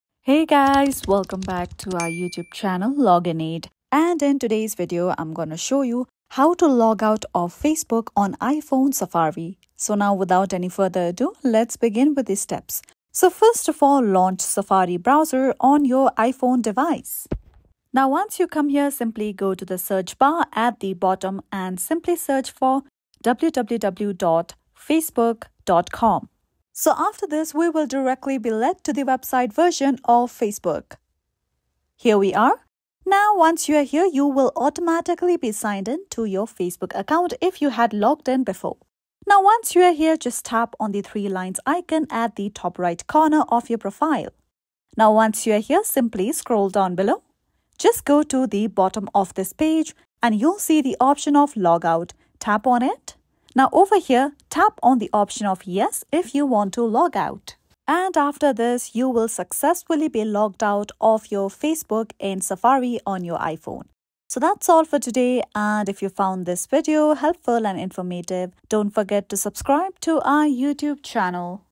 How To Logout Facebook On iPhone Safari Browser 2024 | Facebook Account Sign Out Tutorial
Learn how to easily logout of your Facebook account on iPhone Safari with our tutorial. Logging out of Facebook is essential for privacy and security, especially when using shared devices or public browsers. In this video, we'll guide you through the logout process on Safari, ensuring that your Facebook account remains secure. Take control of your online presence and logout safely from Facebook on iPhone Safari!